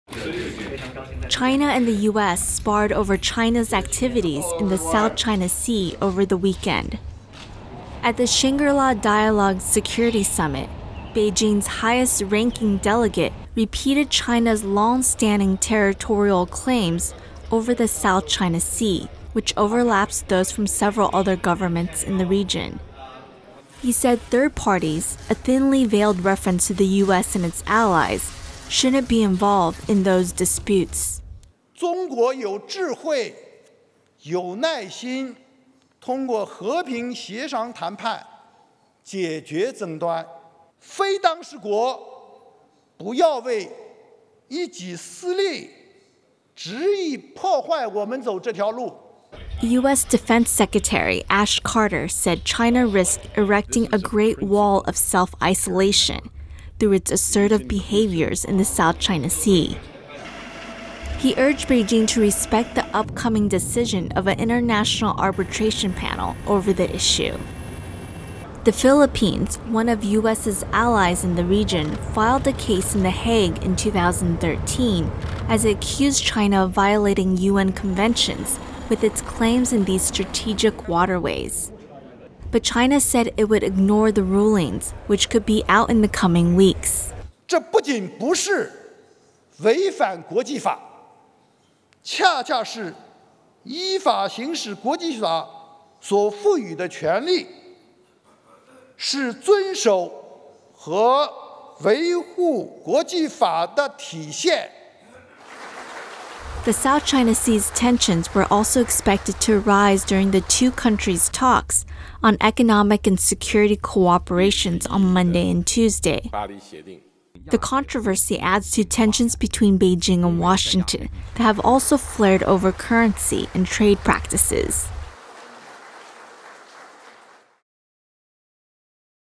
China, U.S. Spar Over South China Sea Disputes
A high-ranking Chinese official spoke stridently against U.S.-led criticism of China’s activities in the South China Sea, ahead of economic and security talks between the two countries in Beijing which began Monday. Photo: Reuters